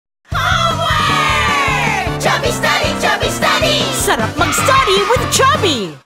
Chubby Study Buddy 6s
Ang homework time, yummy and exciting basta may creamy and chewy study buddy-- Chubby!

ASC R0183P062923C